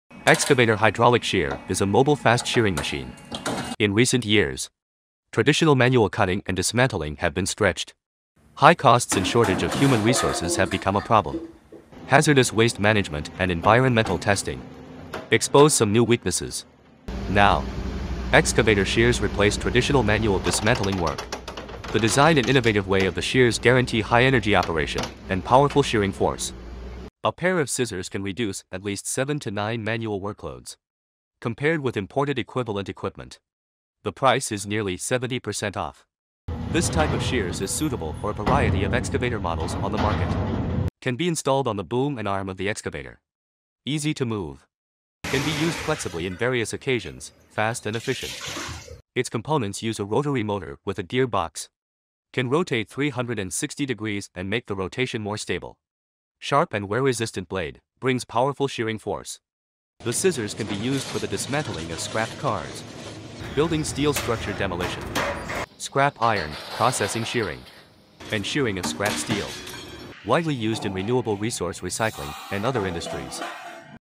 A mobile fast shearing machine
Excavator shears replace traditional manual dismantling work
A pair of scissors can reduce at least 7-9 manual workloads
Widely used in renewable resource recycling and other industries